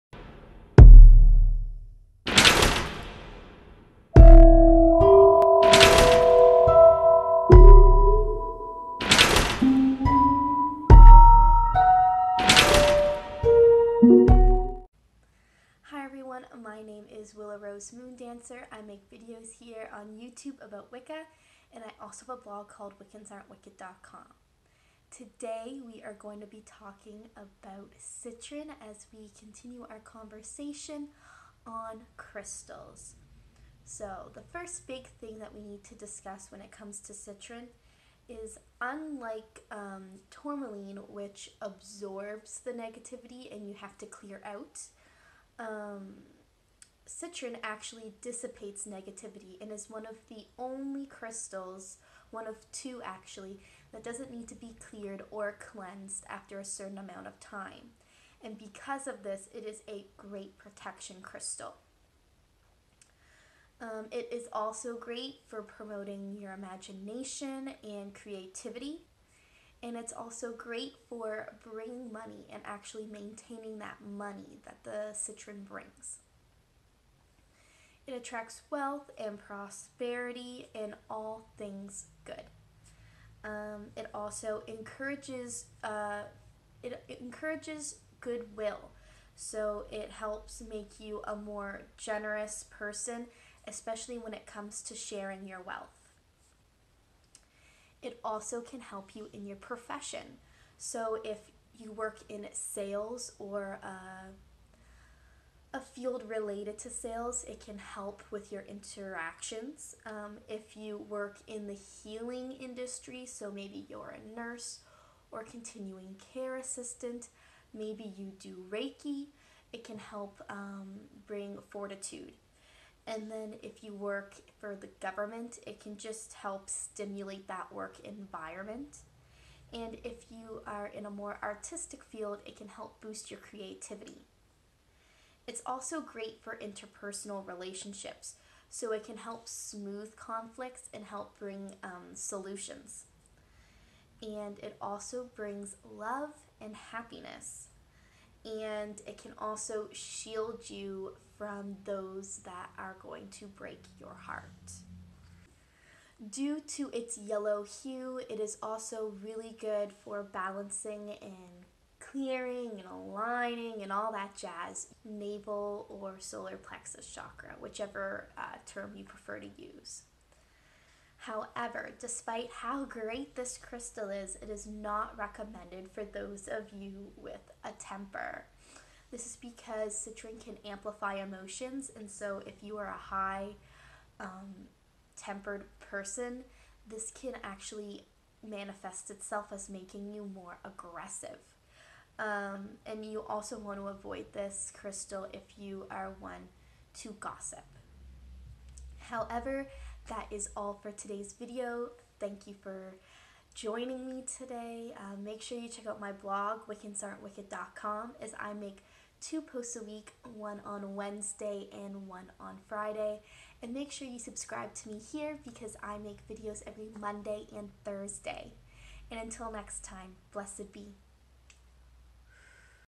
How to use Citrine//Crystal Basics for Beginners
Thanks for checking out today's video! Today we talked about citrine and what it can bring into your life.